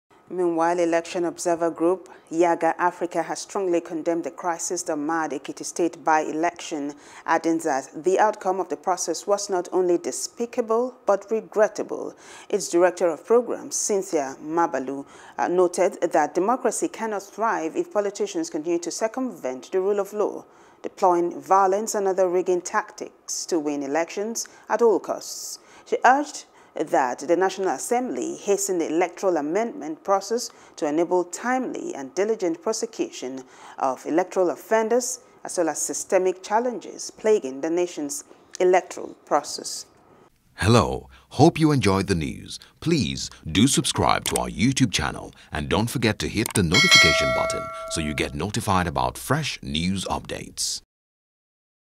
Yiaga Africa Condemns Violence In Ekiti Elections | NEWS
Election observer group, Yiaga Africa, has strongly condemned the crisis that marred Ekiti State bye-election, adding that the outcome of the process was not only despicable but regrettable.